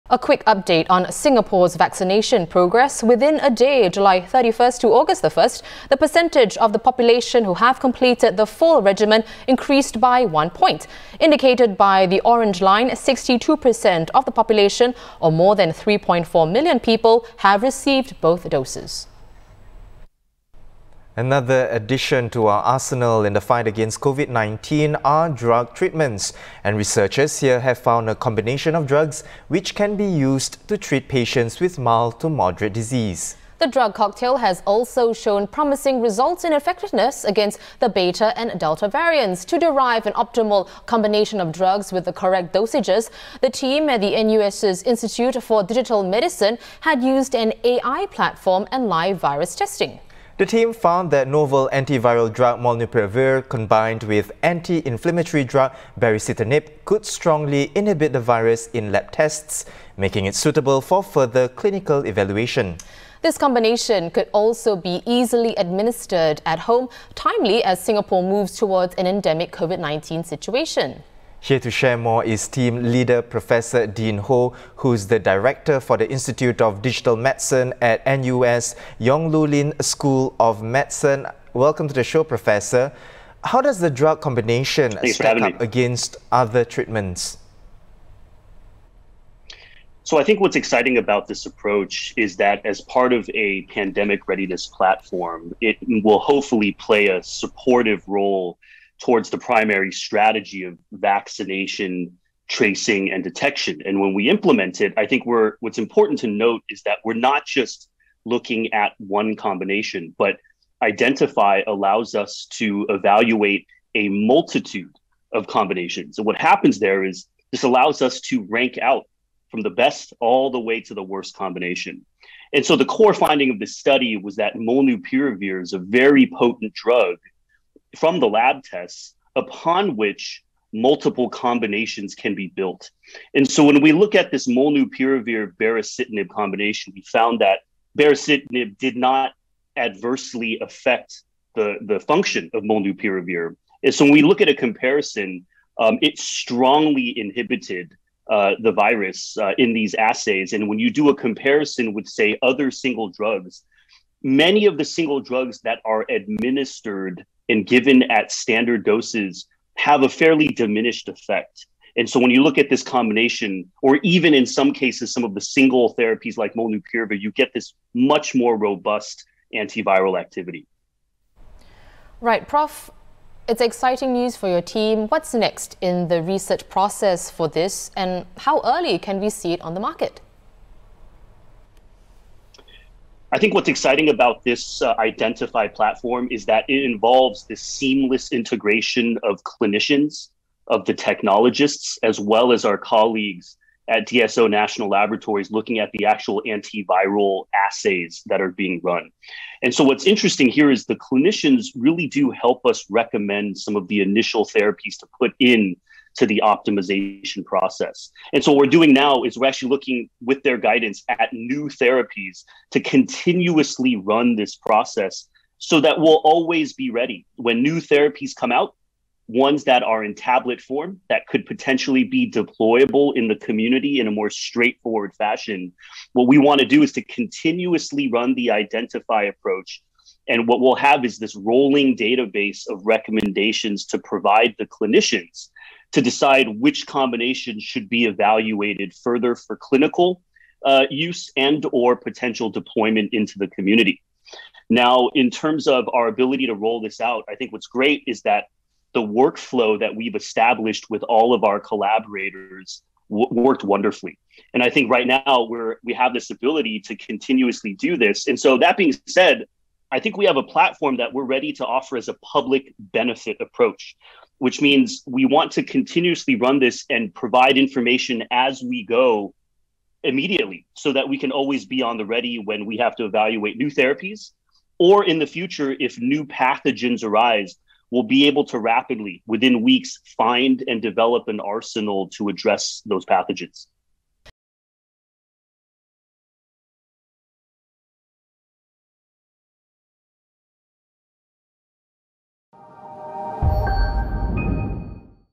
Covid-19: Singapore team finds drug cocktail effective against Delta variant | THE BIG STORY
Researchers in Singapore have found a combination of drugs which can be used to treat Covid-19 patients with mild to moderate disease. The drug cocktail has also shown promising results in terms of effectiveness against the Beta and Delta variants of the virus. Team leader Professor Dean Ho shares more. Prof Ho is the director of the National University of Singapore's Institute for Digital Medicine. Read the story